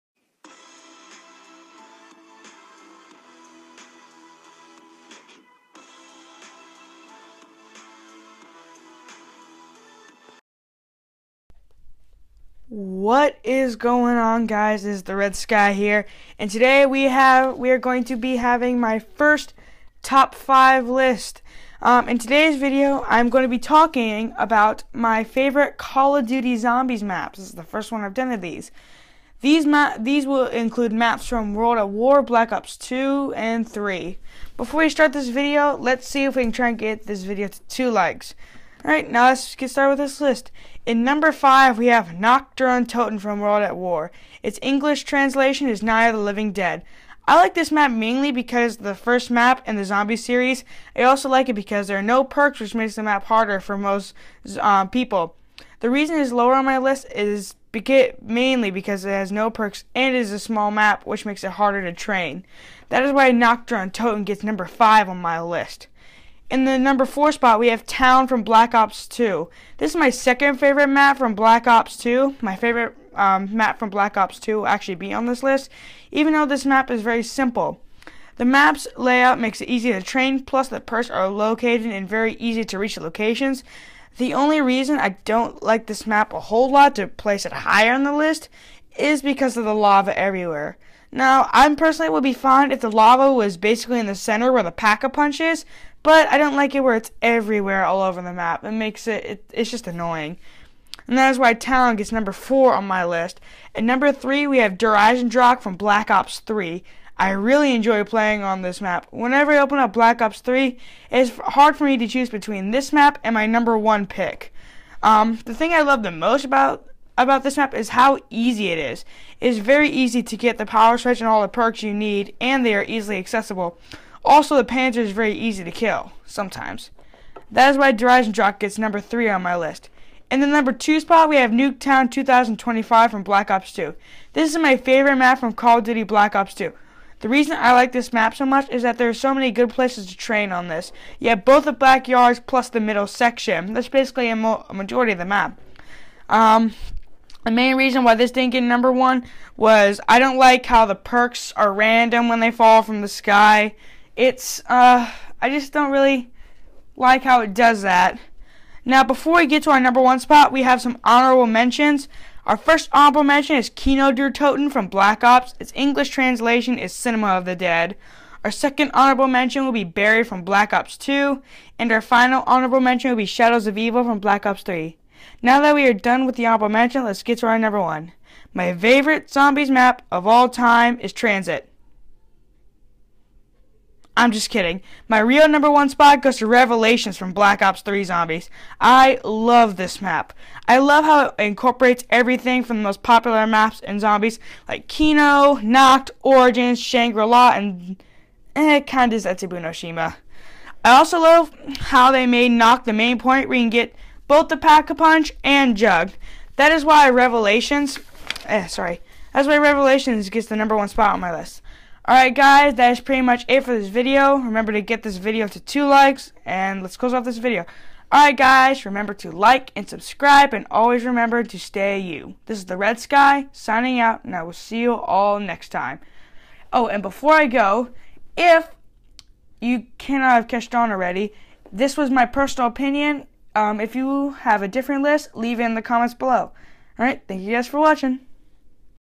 TOP 5 CALL OF DUTY ZOMBIES MAPS (PERSONAL OPINION)
In this video, I tell you guys my top 5 favorite Call of Duty Zombies Maps. Make sure to get this video to 2 likes